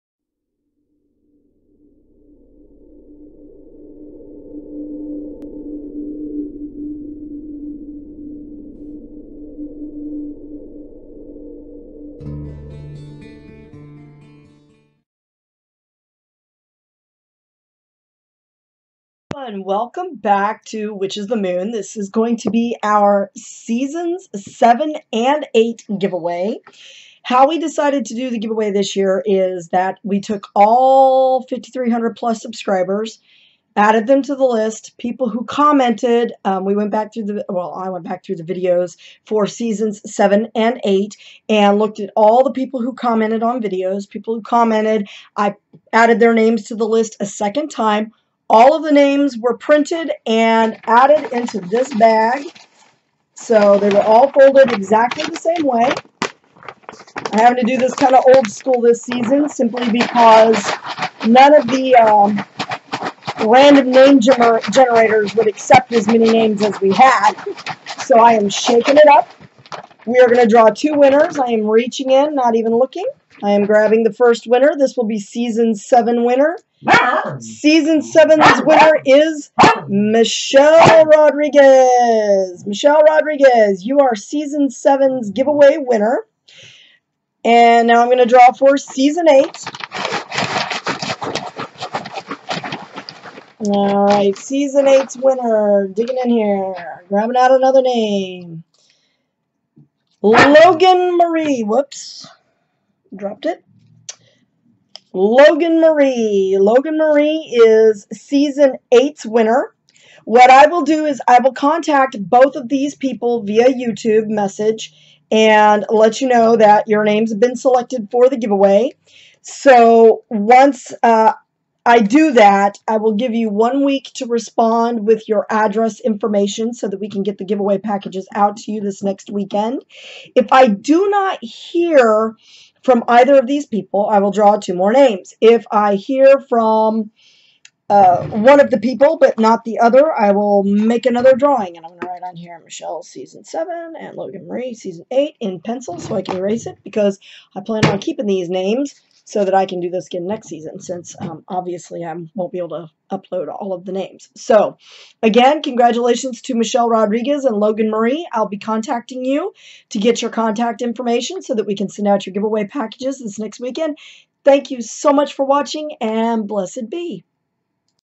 *GIVEAWAY* Seasons 7 and 8!!!!
These are the lucky winners for Seasons 7 and 8!!! If we don't hear from you, we will draw another winner!